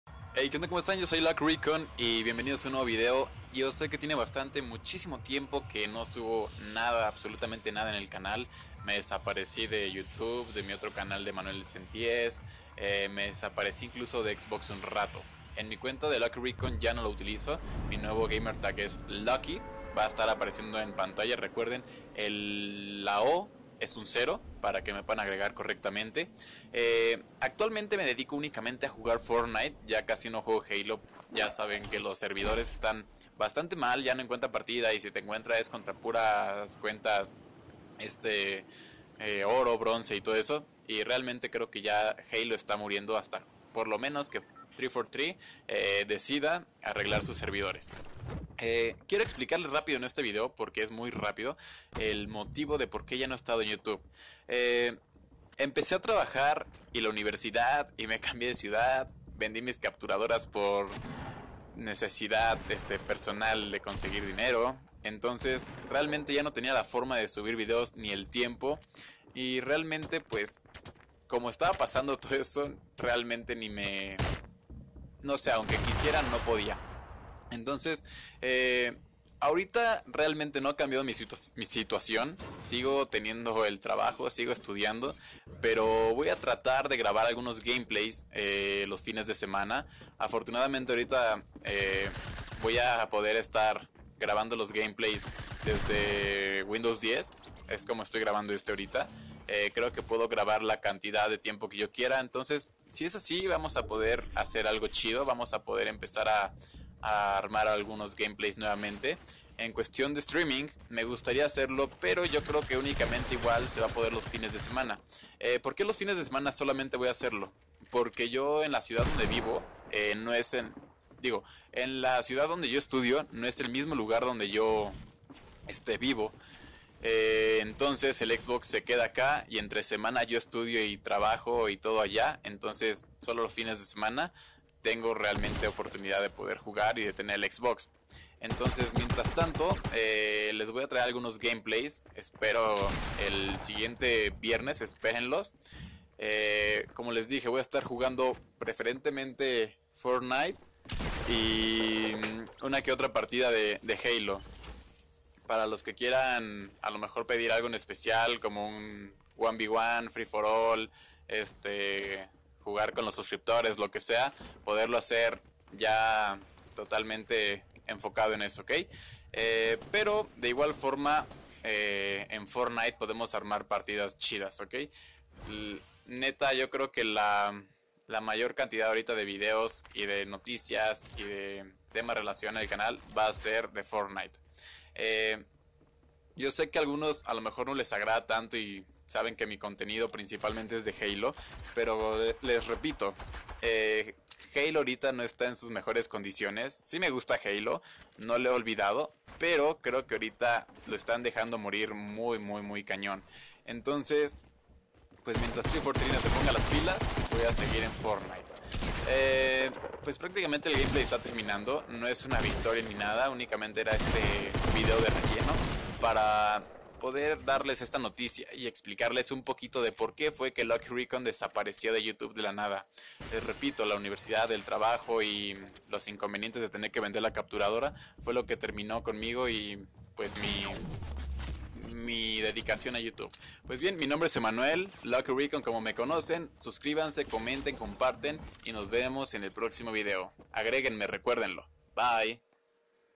Algunos se enojarán
Amigos, breve explicación de mi desaparición tan repentina en YT, estamos de vuelta.

Xbox: L0ckii

Para contribuciones o donación via paypal, cualquier donación será muy bien recibida muchas gracias.